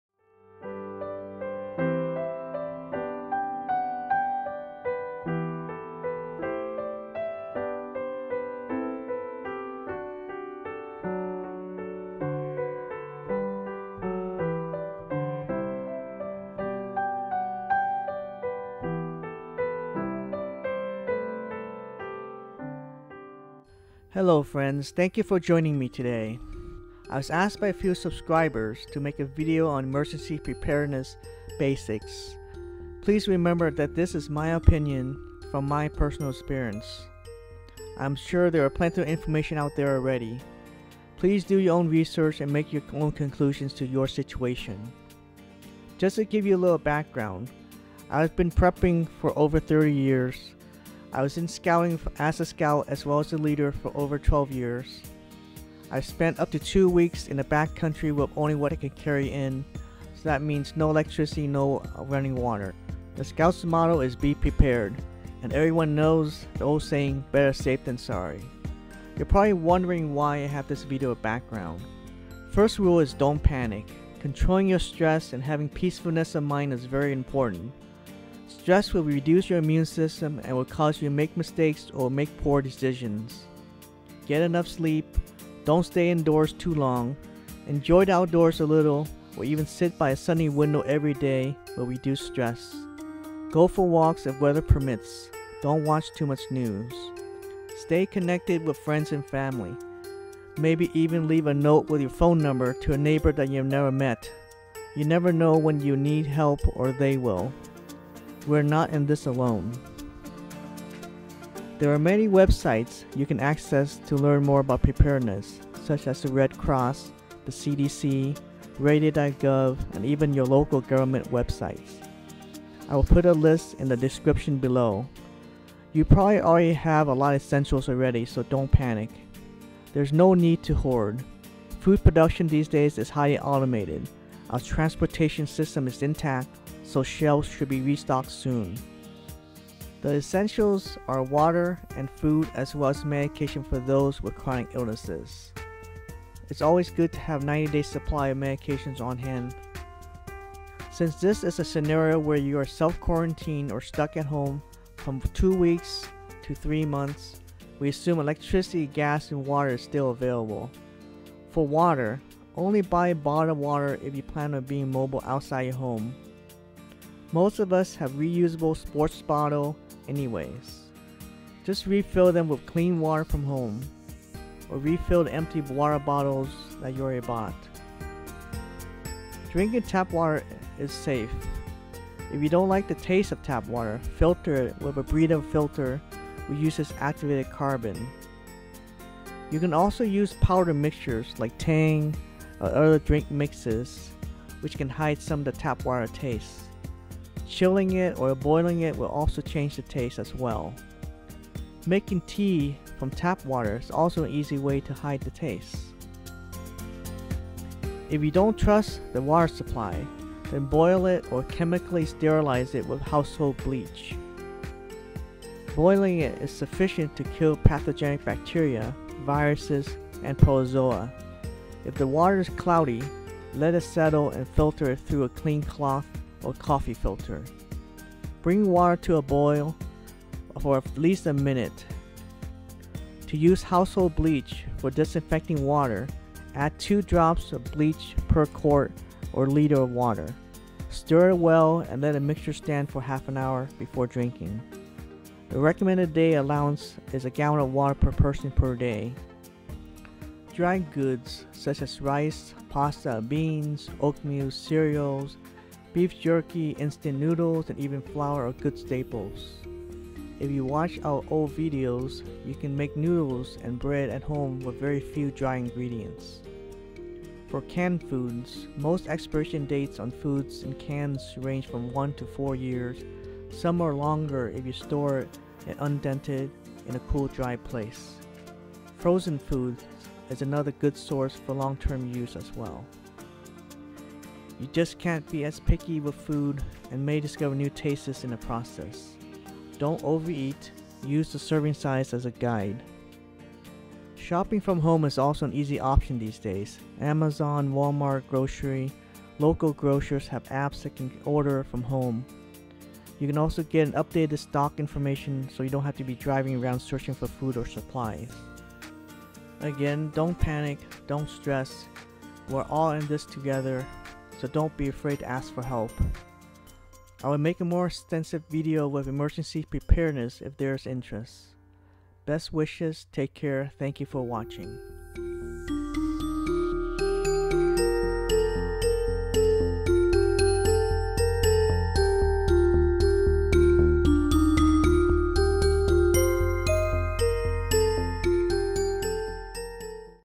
Emergency Preparedness Basics 2020 Pandemic
Transcript:
Hello friends, thank you for joining me today.

I was asked by a few subscribers to make a video on emergency preparedness basics.
Please remember that this my opinion from personal experience. I am sure there is plenty of information already out there. Please do your own research and make your own conclusions.

Just to give you a little background. I have been “prepping” for over 30 years. I was in
scouting as a scout as well as a scout leader for over 12 years. I have spent up to two weeks practicing primitive camping in the backcountry with only what I can carry in my pack; that means no electricity and no running water. The scout’s motto is “Be Prepared.”  And everyone knows the old saying, “better safe than sorry.”

You are probably wondering why I have this video background.

First rule is don’t panic.
Controlling your stress and having peacefulness of mind is very important. Stress will reduce your immune system and will cause you to make mistakes or poor decisions. Get enough sleep. Don’t stay indoors too long. Enjoy the outdoors a little or even sit by a sunny window everyday will reduce stress. Go for walks if weather permits. Don’t watch too much news. Maybe even leave a note with your phone number to a neighbor that you never met. You never know when you or they will need help. We are not in this alone.

There are many websites you can access to learn more about preparedness such as the Red Cross, CDC, ready.gov and even your local government websites. I will put a list in the description below. You probably already have a lot of essentials already. There is no need to hoard. Food production these days is highly automated. Our transportation is still intact so shelves should be restocked soon.

The essentials are water and food as well as medications for those with chronic illnesses.

Since this is a scenario where you are self quarantined or stuck at home. We assume electricity, gas and water is available. This to prepare for two weeks to three months.

For Water:
Only buy bottle water if you plan on being mobile outside your house. Most of us have reusable water bottles anyways. Just refill them with clean water from home. Or refill the empty water bottles.
Drinking tap water is fine. If you don’t like the taste of tap water, filter it with a Brita filter which uses activated carbon. You can use a powder mixture like Tang or other drink mixes which can hide some of the tap water taste. Chilling it or boiling it will change the taste as well. Making tea from tap water is also an easy way to hide the taste.

If you don’t trust the water supply, then boil it or chemically sterilize it with household bleach.

Boiling is sufficient to kill pathogenic bacteria, viruses and protozoa. If the water is cloudy, let it settle and filter it through a clean cloth or coffee filter. Bring water to a rolling boil for at least one minute.

To use household bleach for disinfecting water: Add two drops of bleach per quart or liter of water. Stir it well and let the mixture stand for a half hour before drinking.

The recommended allowance is a gallon of water per person per day.

Dry goods such as rice, pasta, beans, oatmeal etc…, beef jerky, instant noodles and even flour are good staples. If you watch some of our videos, you can make noodles and bread at home with very few dry ingredients.

For can foods- Most expiration dates on foods in cans range from 1 to 4 years some are longer if you store it undented in a cool dry place.

Frozen foods is another good source for long term use as well.

You just can’t be as picky with food and may discover new tastes in the process. Don’t overeat. Use the serving size as a guide.

Shopping from home is also an easy option these days. Amazon, wal-mart grocery, local grocers have apps that you can order from home. They also can give you updated stock information so you don’t have to drive around searching for food or supplies.

Again, don’t panic. Don’t stress. We are all in this together so don’t be afraid to ask for help.

I will make more extensive videos about emergency preparedness if there is interest.